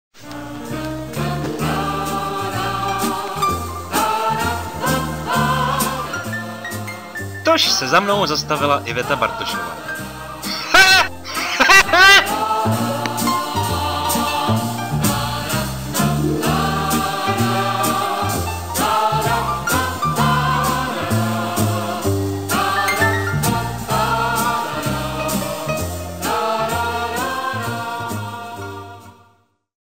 Vladimír Menšík - Bartošová
Vladimír Menšík vypráví smutný příběh s veselým koncem.